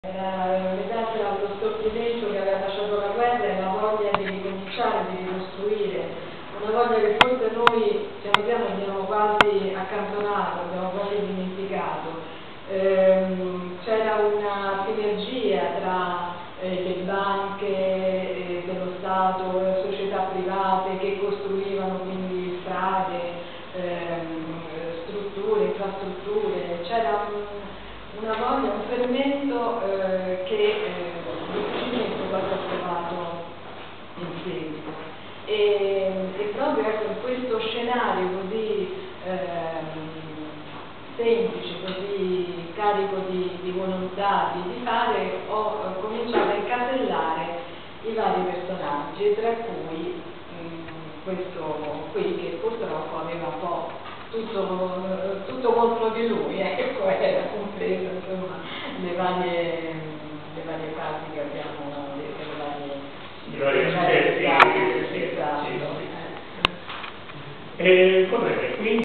"Una morte sola non basta" Presentazione a Roma presso Coaster
Risposta alle interessanti domande del relatore, lo scrittore bruno brundisini